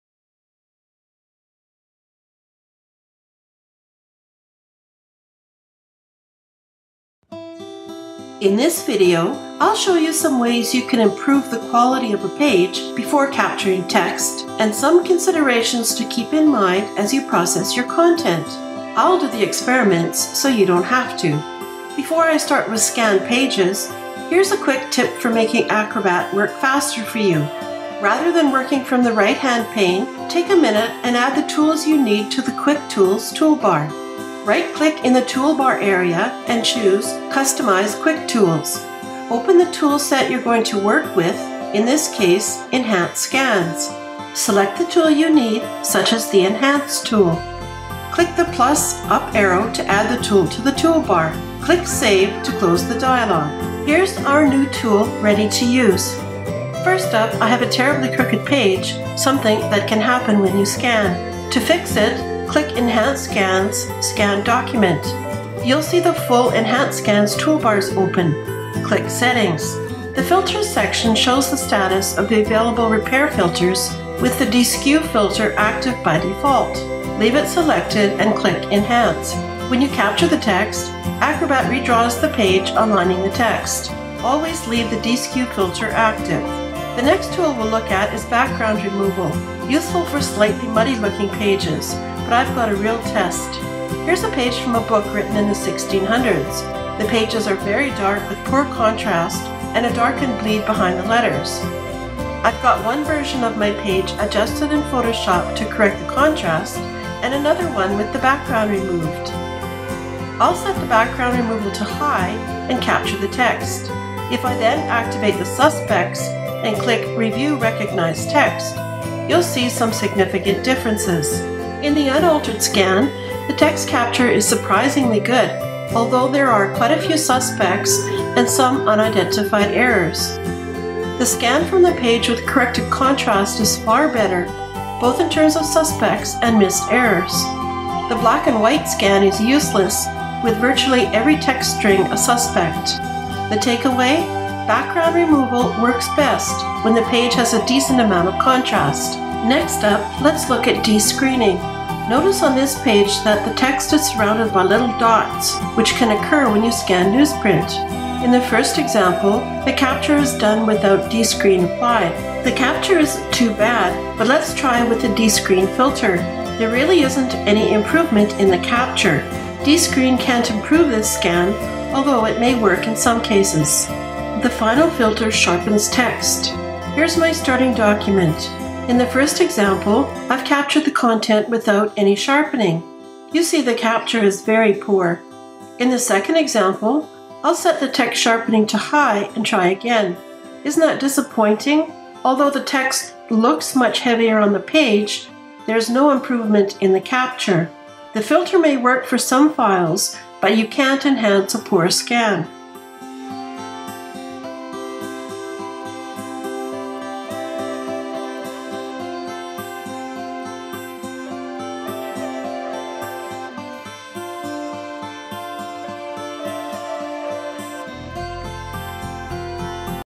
Acrobat DC: Enhancing Scanned Page Contents
See how to use the Enhance Scan filters